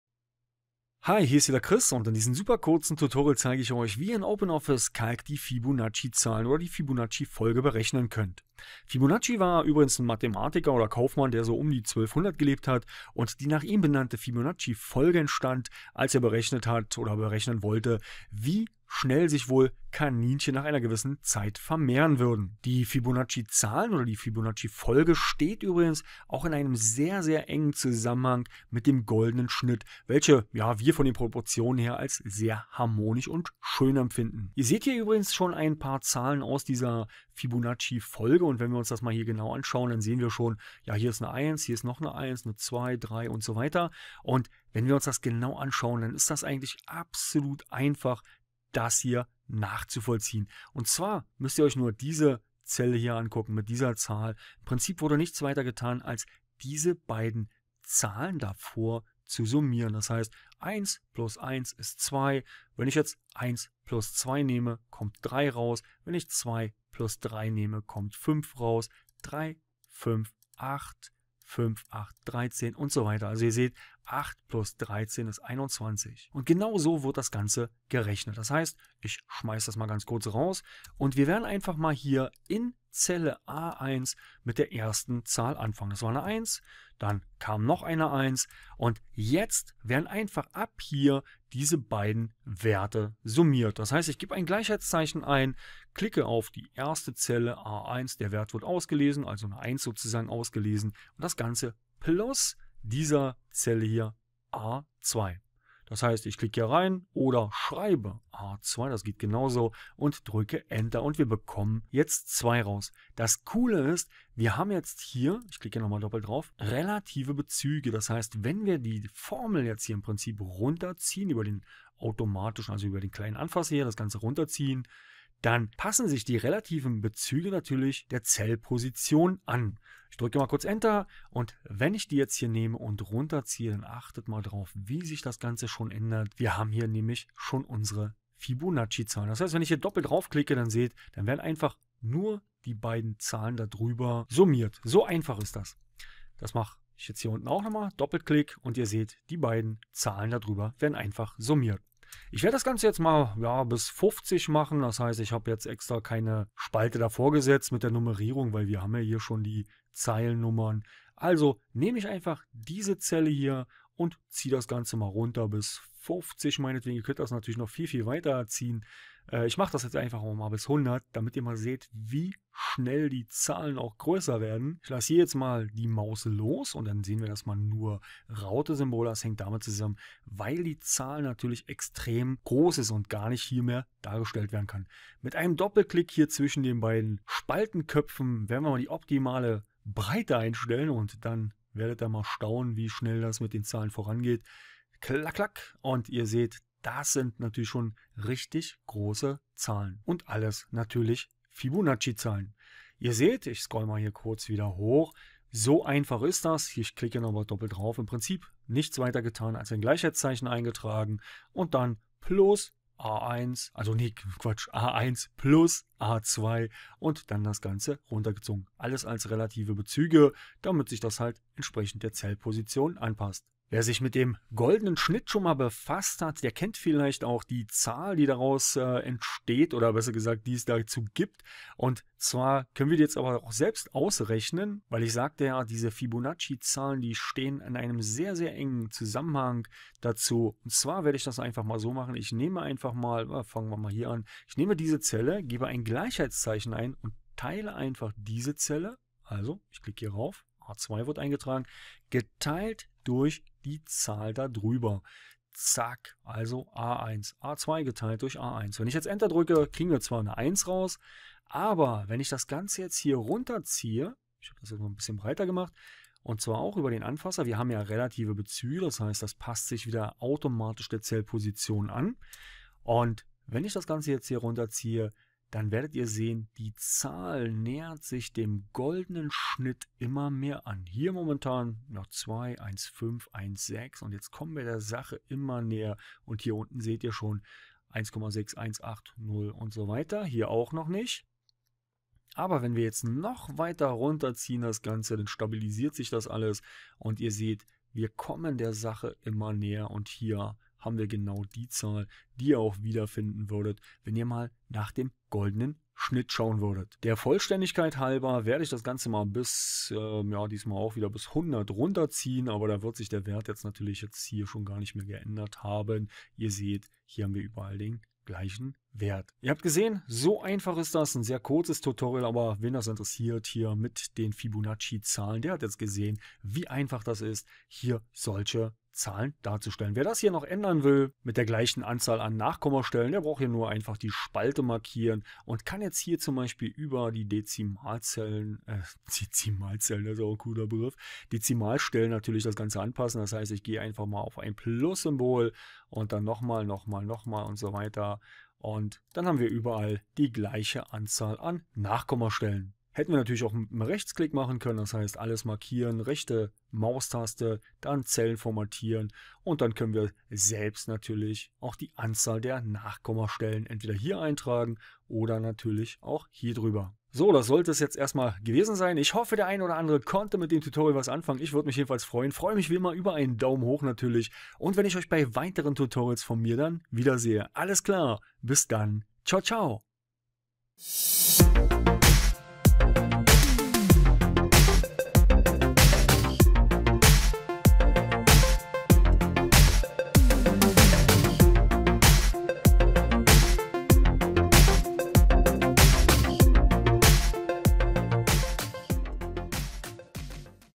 Fibunacci Folge berechnen (OpenOffice Calc)
Hier meine gesamte OpenOffice Calc Playlist (mehr als 100 Videos)
In diesem Tutorial zeige ich Euch wie Ihr in OpenOffice Calc die Fibunacci Folge berechnen könnt.
Die Fibunacci Zahlen oder Fibunacci Folge steht in sehr engem Zusammenhang zu dem goldenen Schnitt, welcher häufig im Bereich der Kunst und der Architektur zu finden ist.
Ein leicht verständliches Tutorial für Anfänger aus dem Bereich:
OpenOffice Calc

Diese Anleitung / Lernvideo ist in deutsch / german.